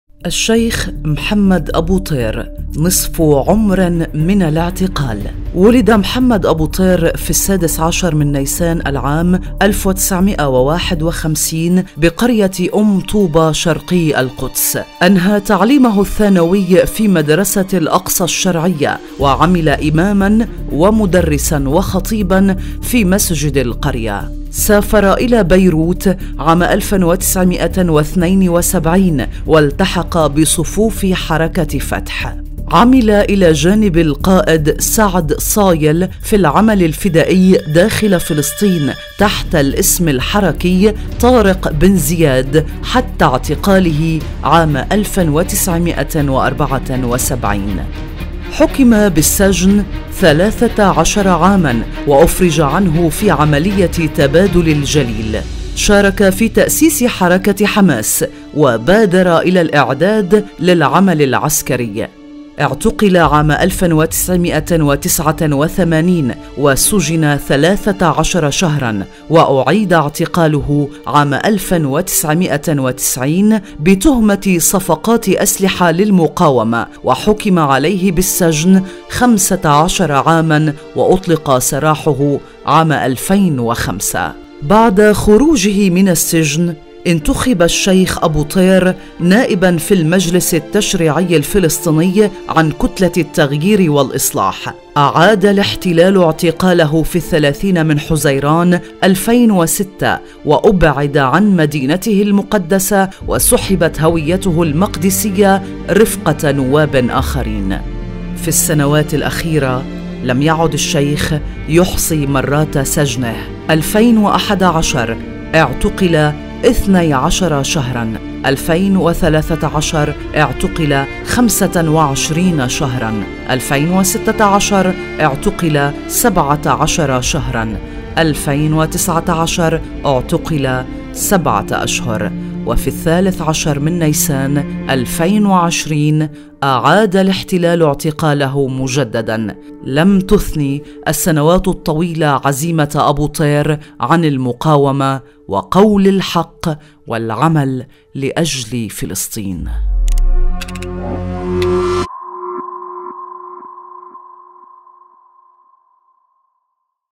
النائب الأسير محمد أبو طير .. قضى أكثر من نصف عمره في سجون الاحتلال
نائب فلسطيني أمضى أكثر من نصف عمره في سجون الاحتلال، وبدأ عمله في المقاومـة إلى جانب الشهــيد سعد صايل في لبنان منذ سبعينيات القرن الماضي...
المزيد عن الشيخ الأسير والنائب في المجلس التشريعي محمد أبو طير مع شيرين موسى حبش